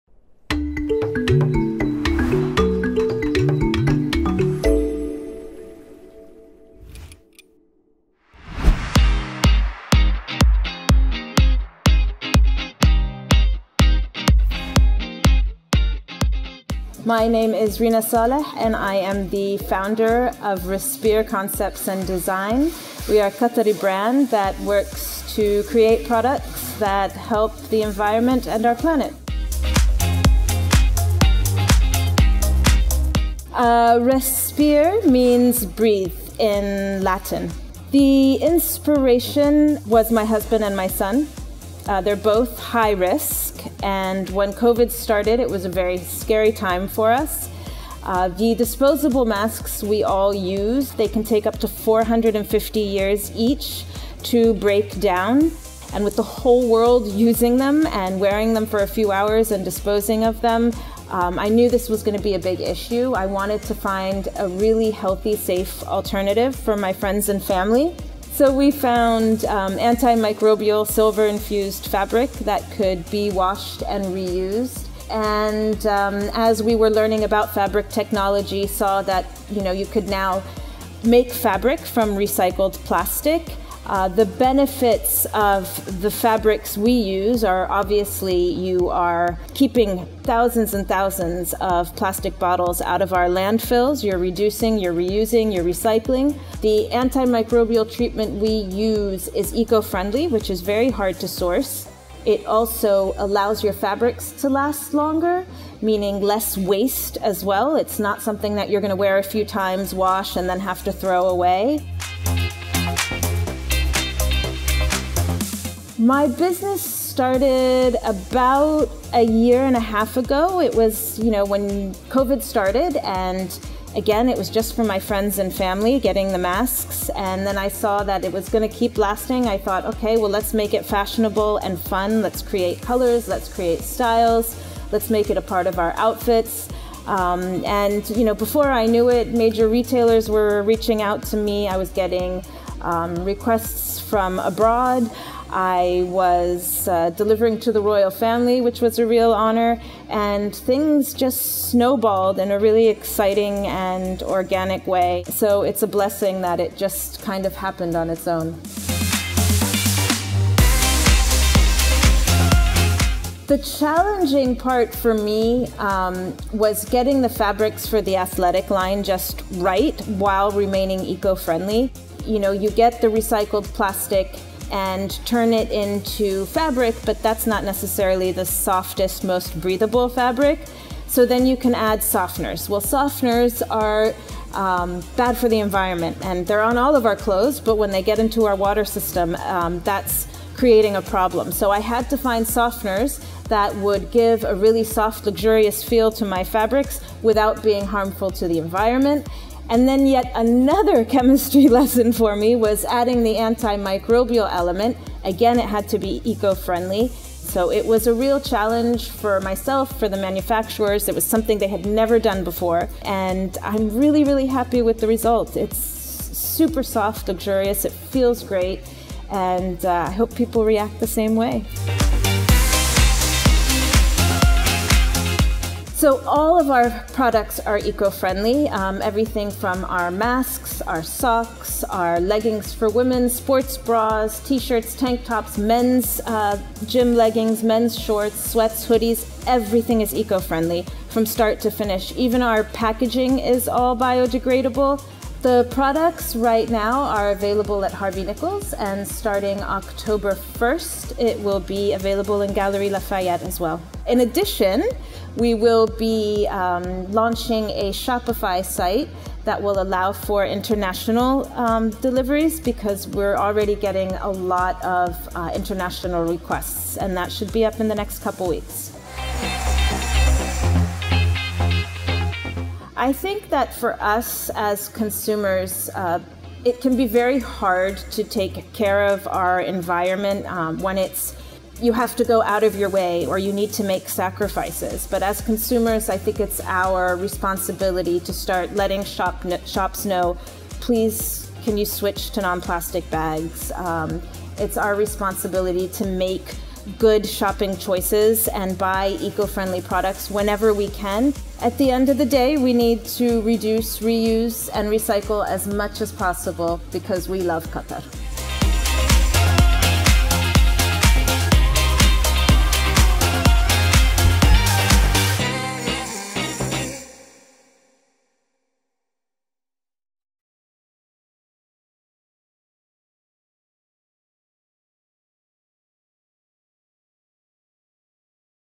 Do you know clothes can be made from recycled plastic bottles? | Live Green | Ep 27
This episode of 'Live Green' is all about clothes, but these are no ordinary clothes. They are RSPR athleisure wear and they're made out of recycled plastic bottles. Yes, you read correctly - recycled plastic bottles!

Rina Saleh tells us how she came up with this unique concept of combining fashion with sustainability to design and produce a whole range of RSPR (pronounced res·pir – Latin for breathe) athleisure and masks that are made with plastic bottles and microbial fabric, so you not only look good but know you're looking after the planet, too!

Do you know each piece of RSPR clothing has a number that depicts how many plastic bottles went into its making?

To find out more about RSPR and how you can get in touch with them, visit their social media page:

Doha, Qatar